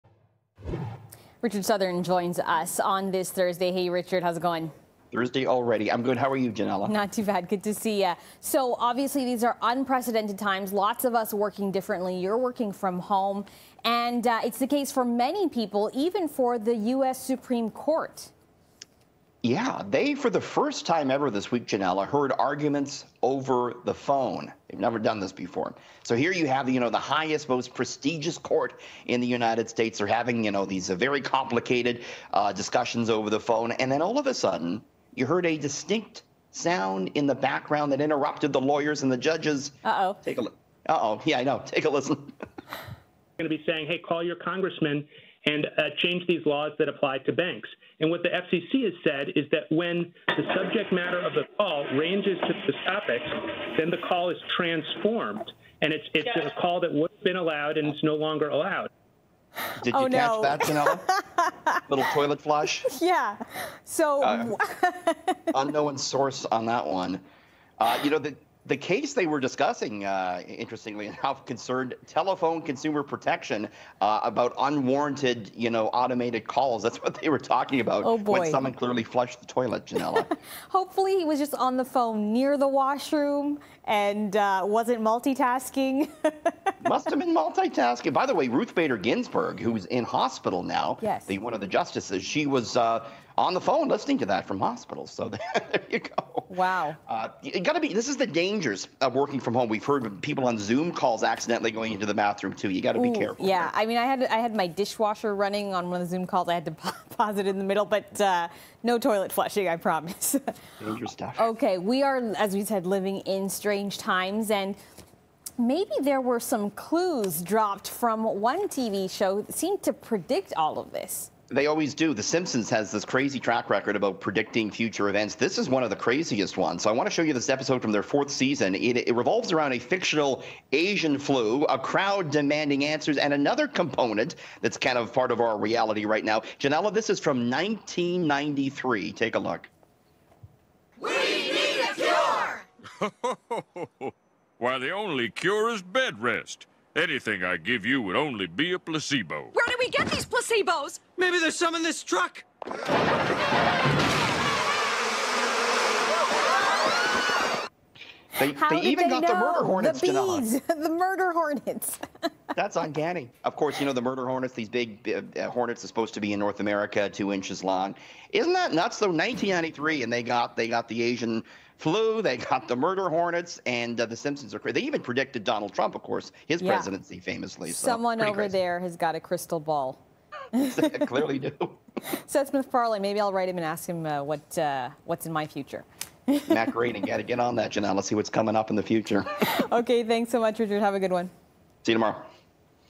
Flushing toilet heard during U.S. Supreme Court hearing
A flushing toilet was heard during a recent U.S. Supreme Court telephone hearing. Plus, with pandemic fears and murder hornets buzzing around, it seems The Simpsons may have predicted the future once again. Richard Southern and Ginella Massa discuss.